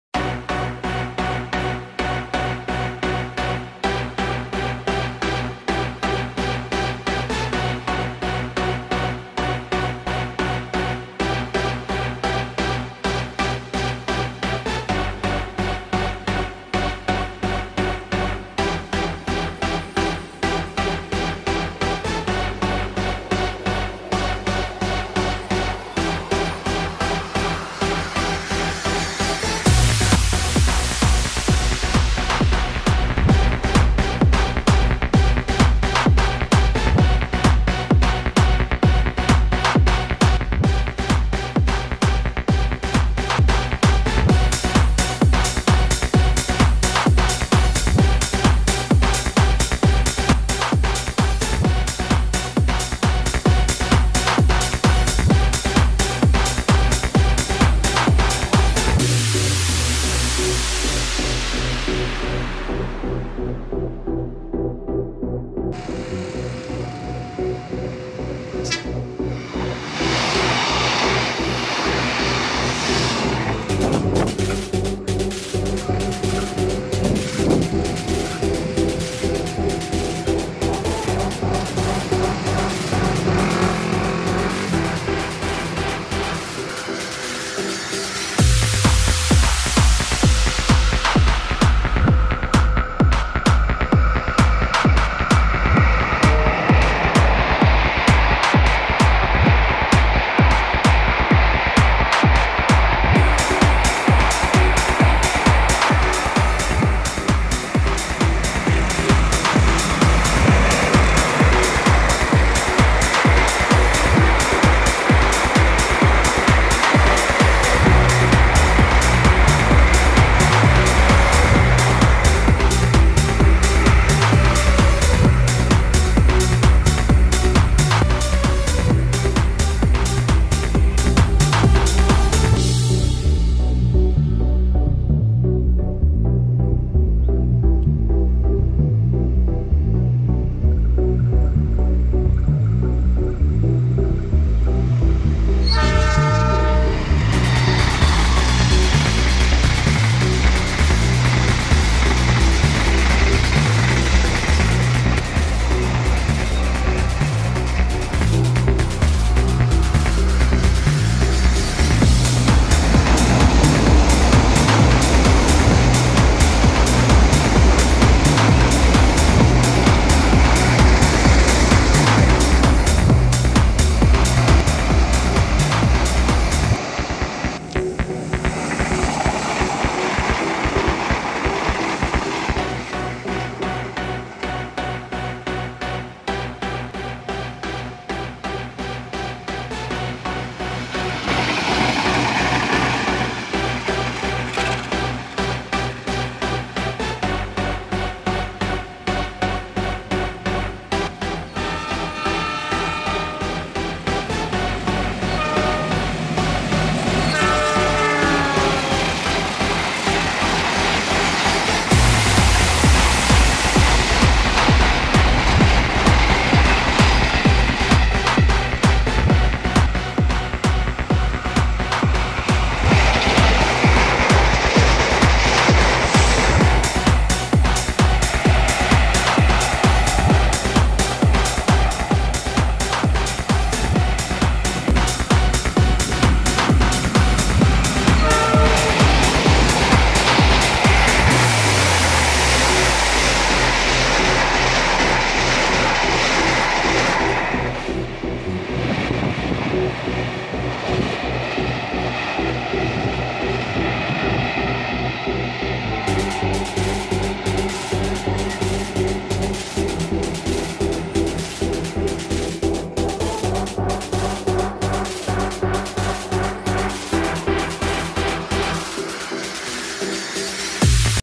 Backsound Music Kereta api || Kompilasi kereta ngebut di St Patukan
Backsound Music Kereta api || Kompilasi kereta ngebut di St Patukan Gamping Sleman .
Kereta api ngebut dengan backsound musik yang keren .

Lokasi video
Stasiun Patukan dan perlintasan JPL 727
Sumber music Youtobe Library
Kereta Melintas
- KA Bandara
- KA Taksaka
- KA Joglosemarkerto
- KA Ranggajati
- KA BBM Pertamina
- KA Argo Wilis
- Lokomotif CC 201

Tonton video saya kali ini ya

Salam sahabat kereta api Indonesia semoga sehat selalu